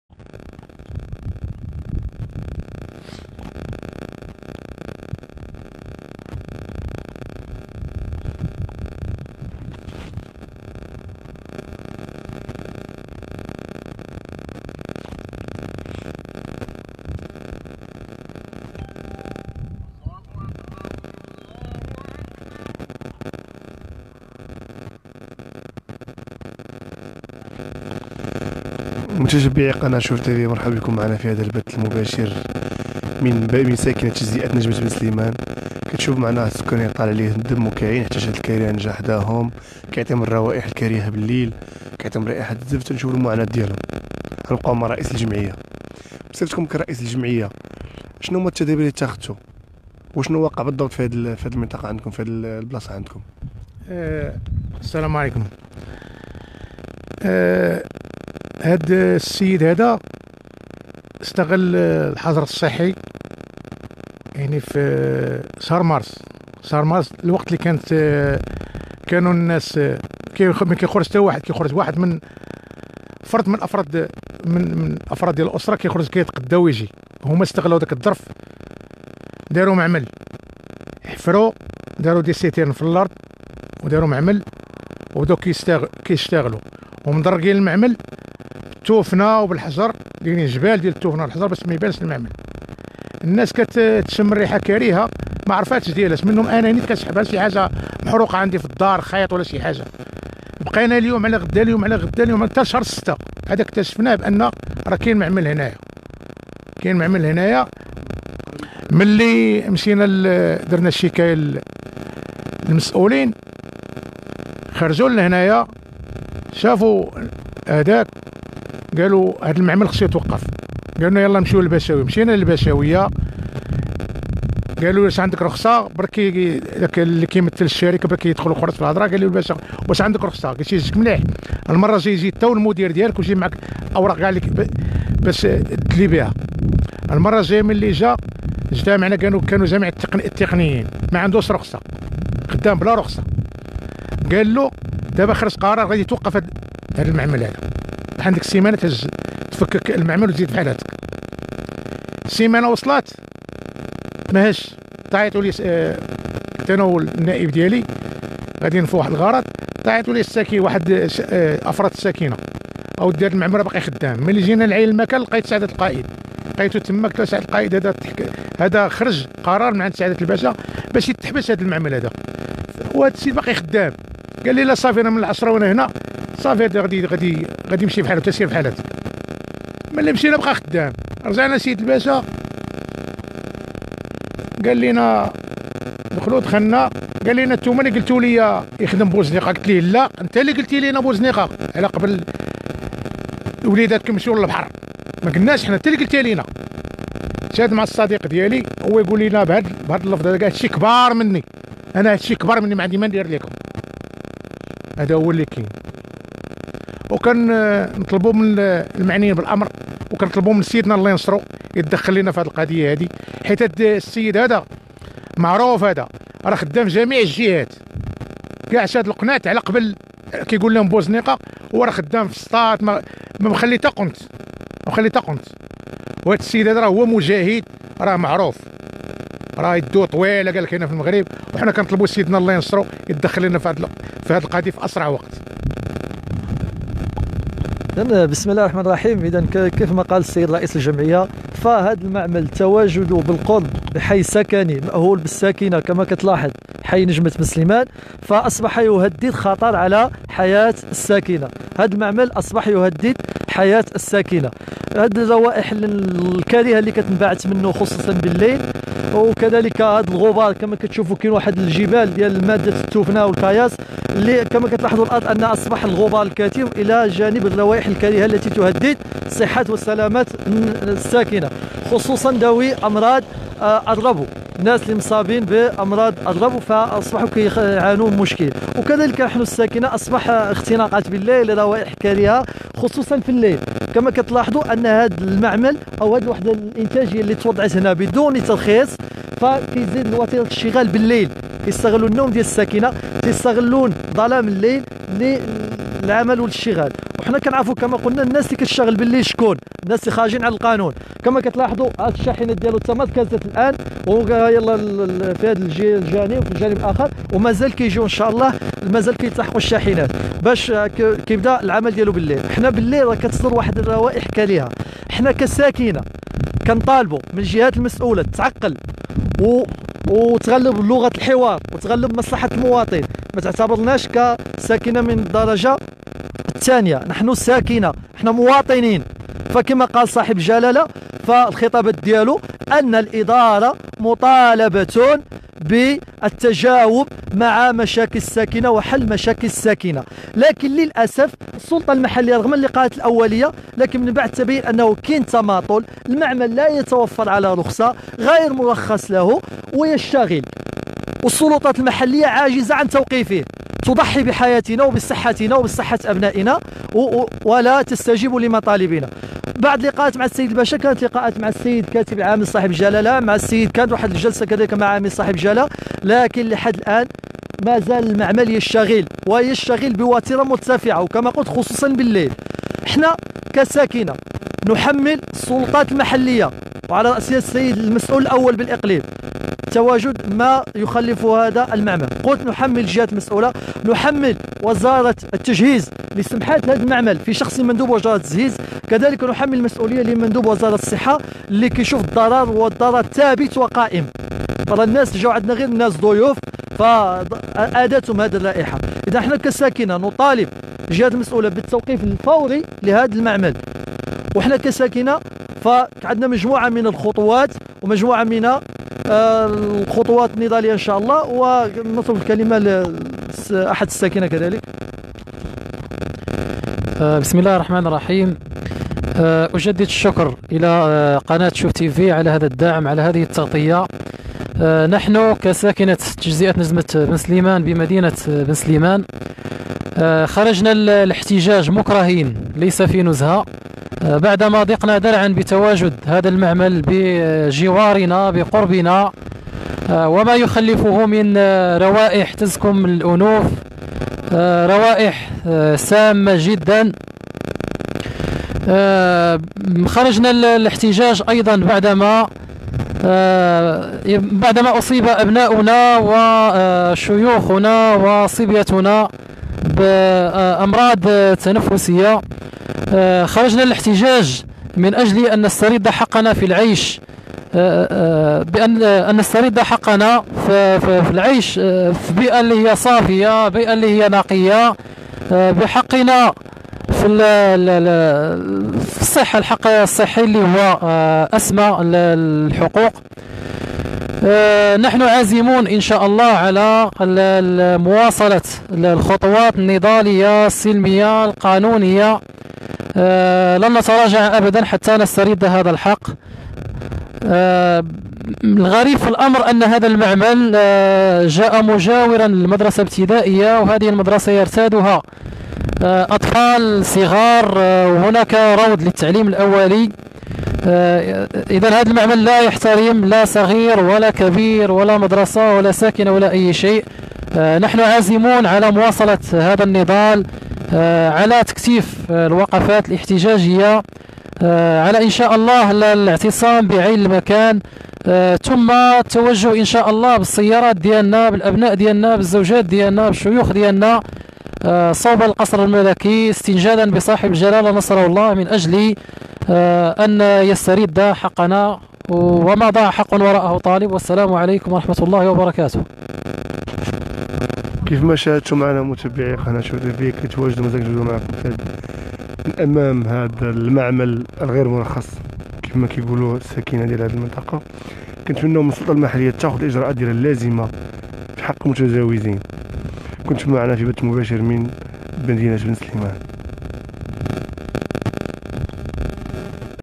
من بنسليمان: ساكنة تجزئة نجمة بنسليمان طالع ليهم الدم بسبب الكاريان لي حداهم"قهرنا بريحة الزفت"
مباشرة من بنسليمان: ساكنة تجزئة نجمة بنسليمان طالع ليهم الدم بسبب الكاريان لي حداهم"قهرنا بريحة الزفت والمسؤولين مامسوقينش لينا"